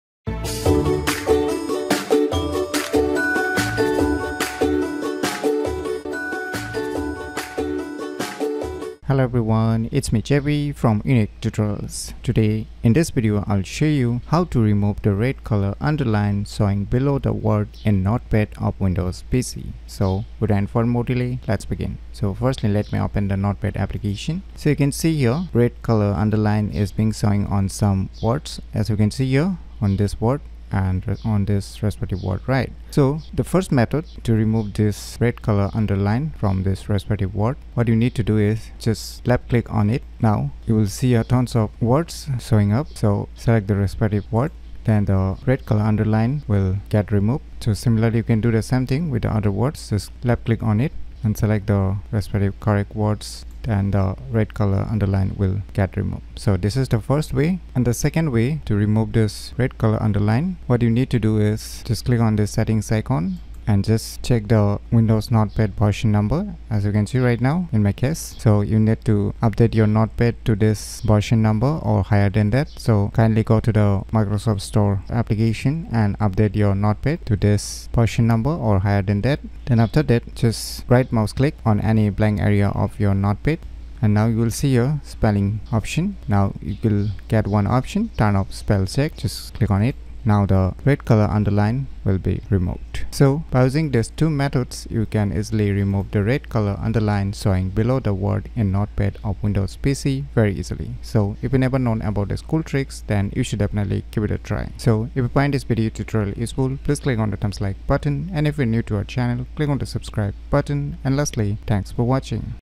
How to remove the " Red Colour Underline " showing below the word in Notepad of Windows PC ?
This video tutorial is all about How to remove the " Red Colour Underline " showing below the word in Notepad of Windows PC.